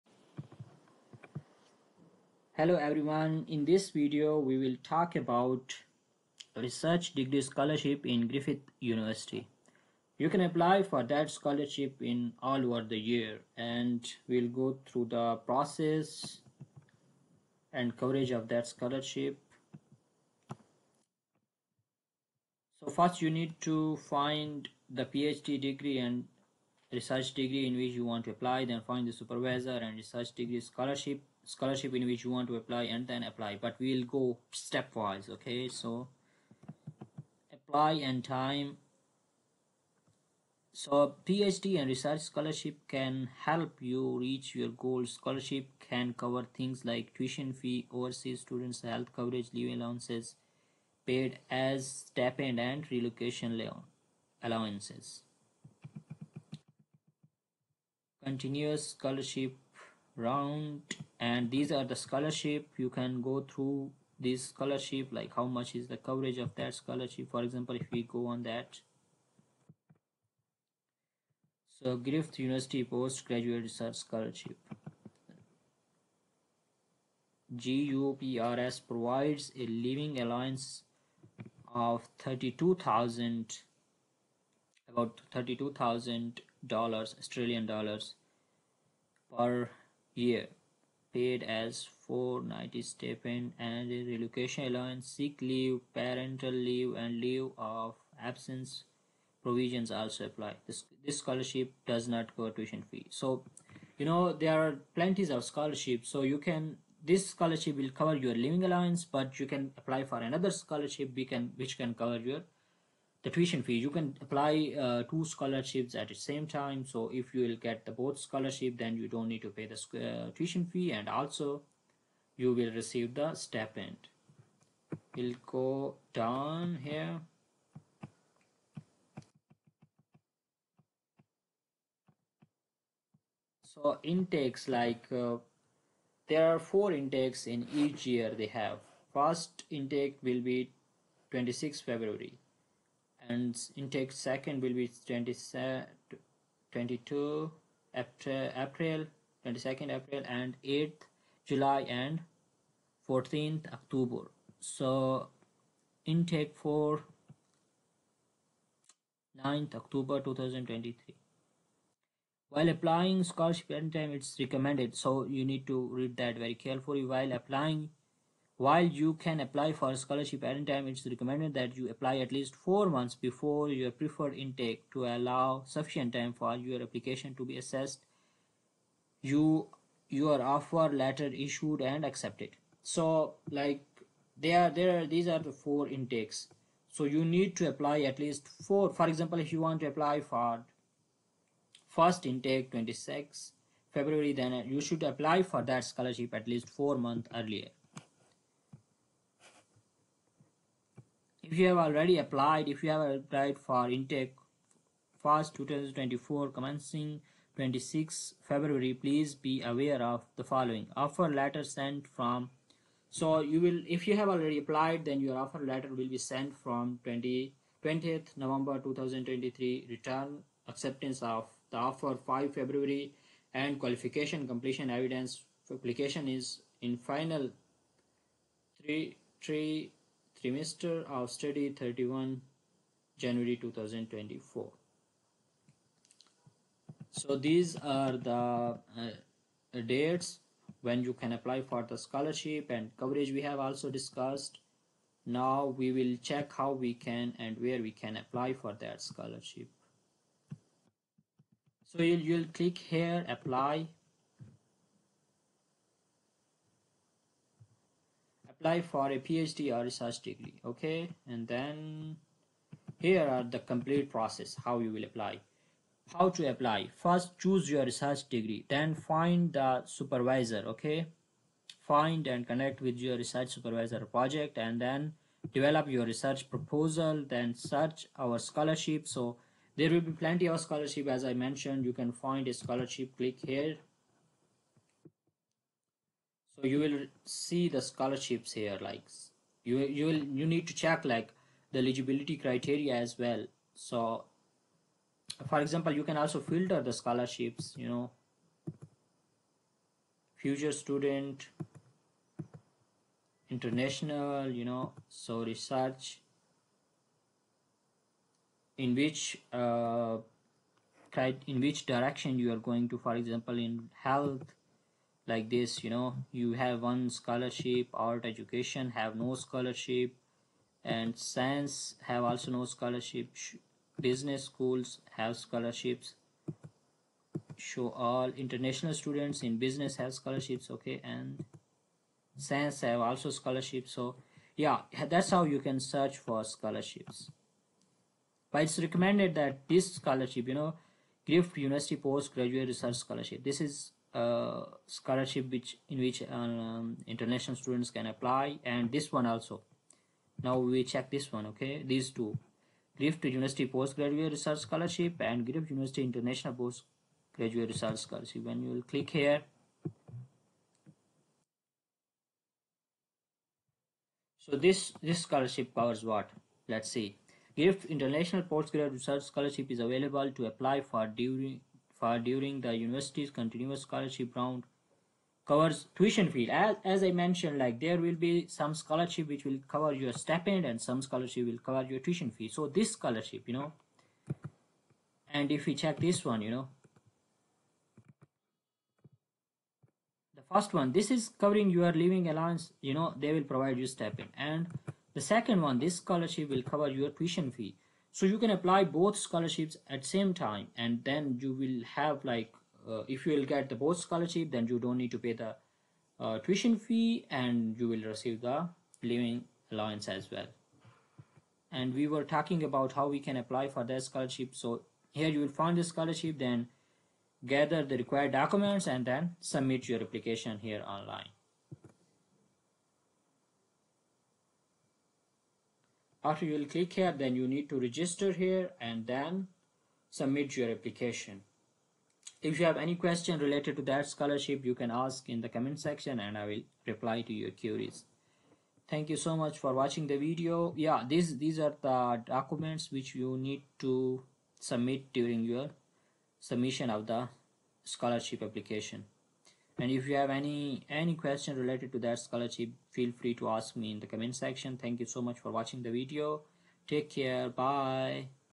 Griffith University Australia Scholarship: Stepwise Procedure
📚 Scholarship Hunters Club 🌟

If you want to pursue your education in Australia and need financial assistance, you've come to the right place. In this video, we'll take you through the step-by-step procedure to apply for scholarships at Griffith University, one of Australia's leading academic institutions. Whether you're an international or domestic student, Griffith University offers a range of scholarships that can make your dream of studying Down Under a reality.

In this comprehensive guide, we'll cover everything you need to know to successfully apply for scholarships at Griffith University. We'll start by discussing the different types of scholarships available, including those for undergraduates, postgraduates, and even specific programs of study. You'll learn about the eligibility criteria, the application process, and key deadlines.

We aim to provide you with the information and insights you need to maximize your chances of securing a scholarship. We'll also share valuable tips and strategies to stand out in the application process and make a compelling case for why you deserve this financial support.

Studying abroad is a life-changing experience, and with Griffith University's scholarships, you can make it a reality. Don't miss out on the opportunity to receive financial assistance to pursue your academic goals and broaden your horizons in Australia.

Thank you for joining us, and let's get started on your path to securing a scholarship at Griffith University in Australia!